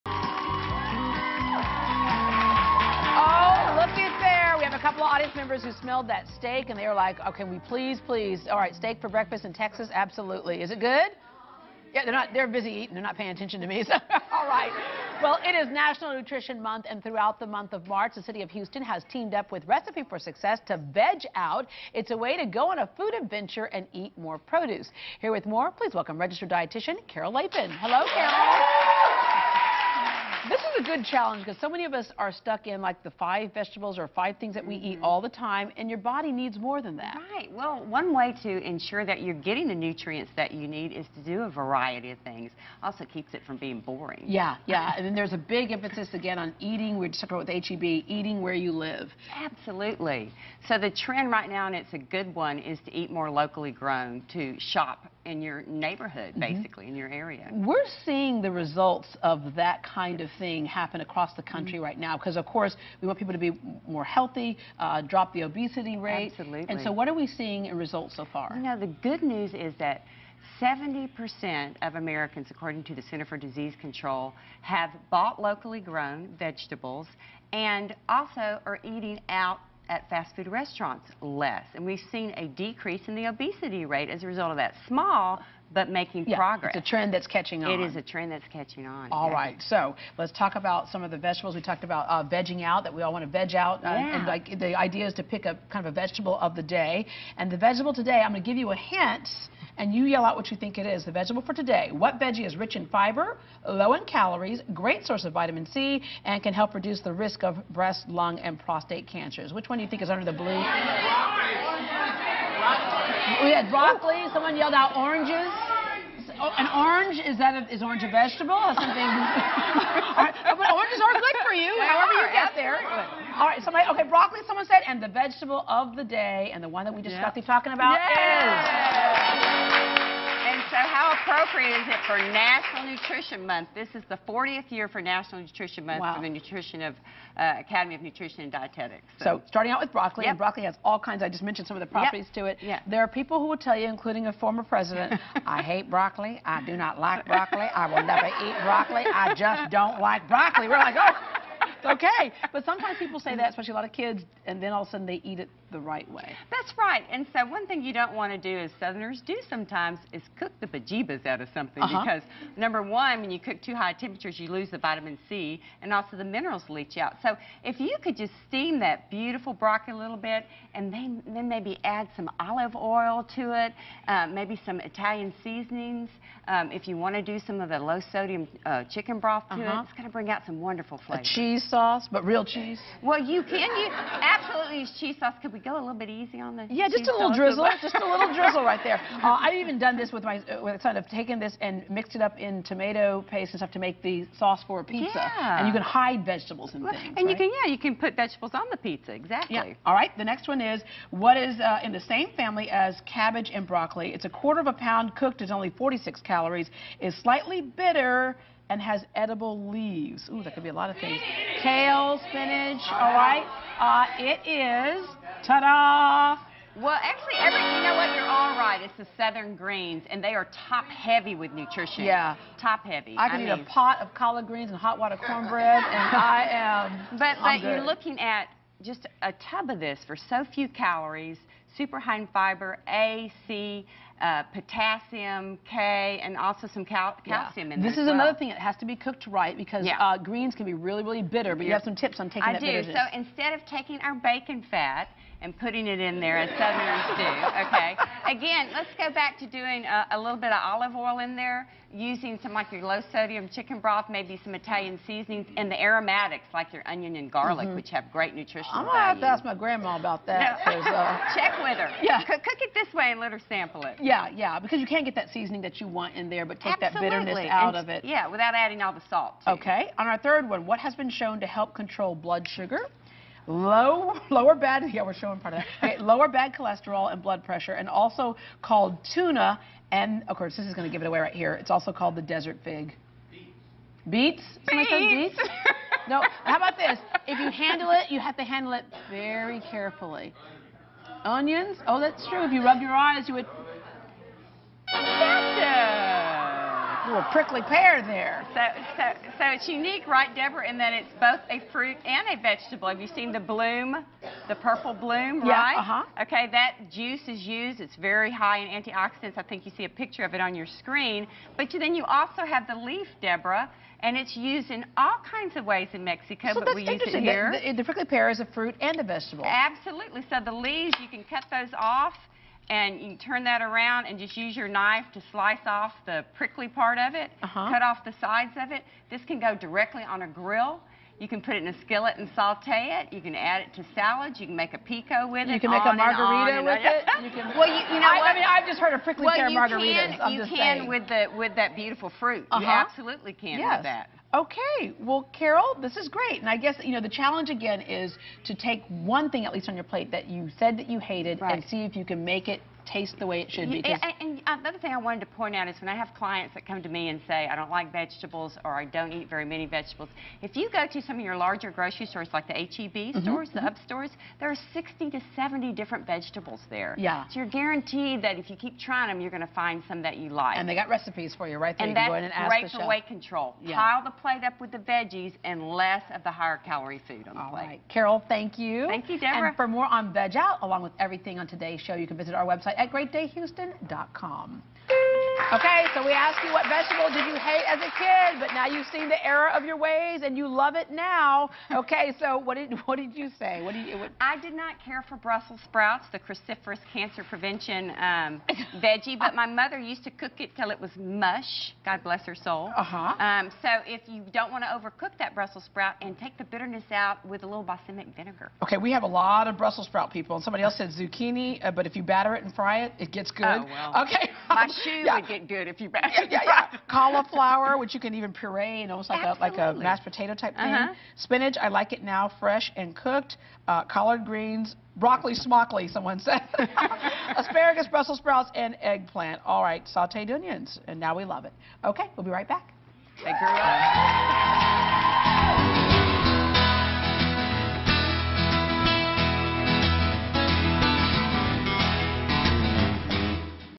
KHOU Says Time to VegOut! with Recipe for Success
Take a food adventure and eat more produce. It's National Nutrition Month and throughout month of March, the City of Houston has teamed up with Recipe for Success to VegOut! Registered dietitian, Carol Lapin is here to explain more.